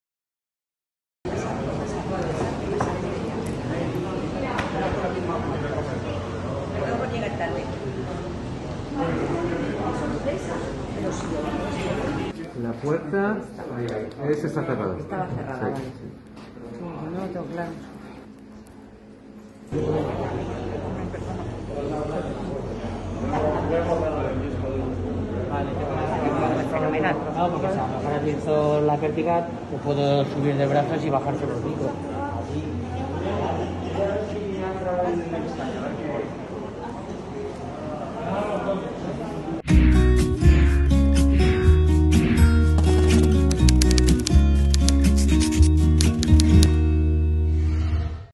Rodaje. Día 2.
Martes 19 de julio.
Escena importante de Carmen Maura con Marisol Ayuso,MªLuisa Merlo y MªJosé Alfonso. ¡Cuatro cracks!